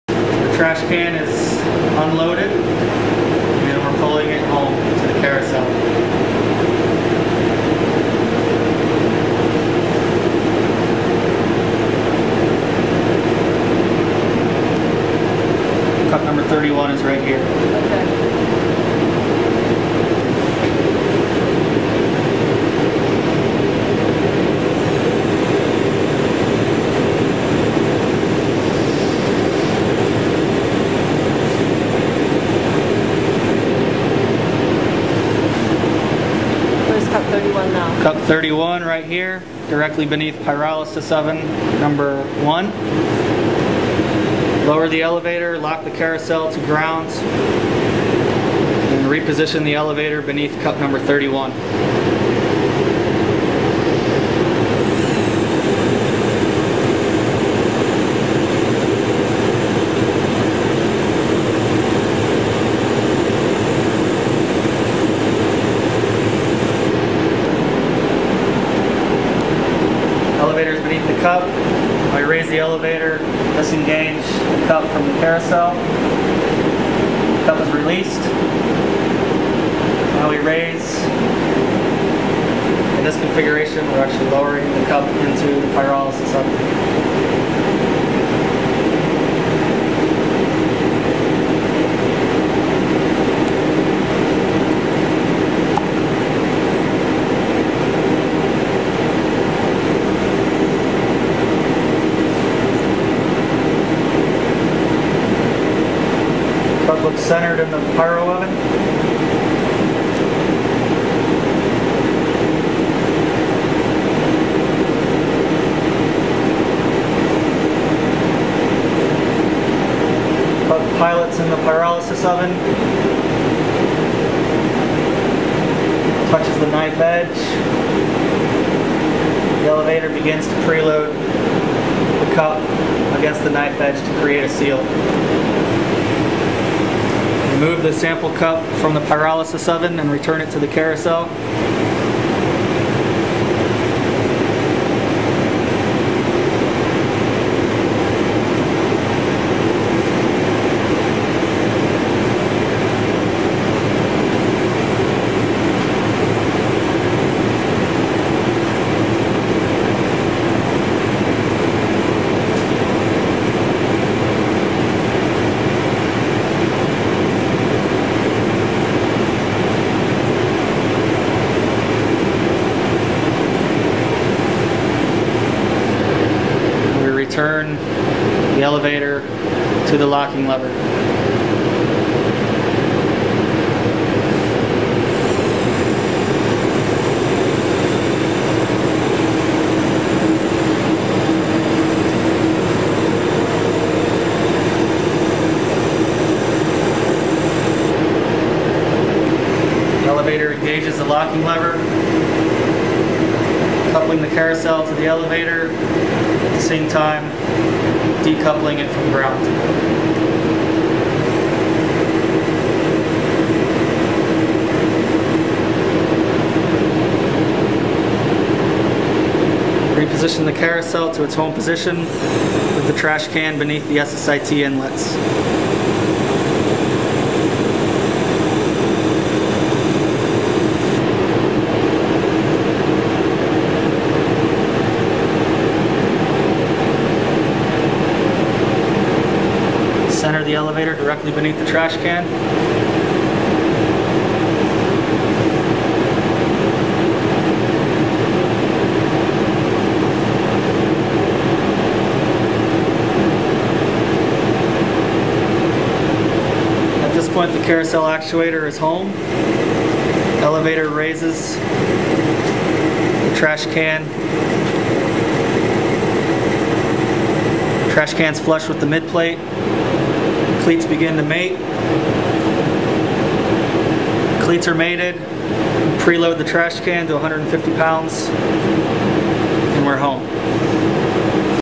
MSL Sample Manipulation System (SMS)
The Sample Analysis at Mars (SAM) suite of instruments is integral to the upcoming Mars Science Laboratory (MSL) mission, which will enable scientists on Earth to remotely examine soil and rock samples for evidence of past and present habitable environments.

The SMS plays an integral role in the path of a sample from the Martian surface to the analytical instruments within SAM. The system provides 74 quartz cells for accepting, storing, and delivering the samples to the science suite.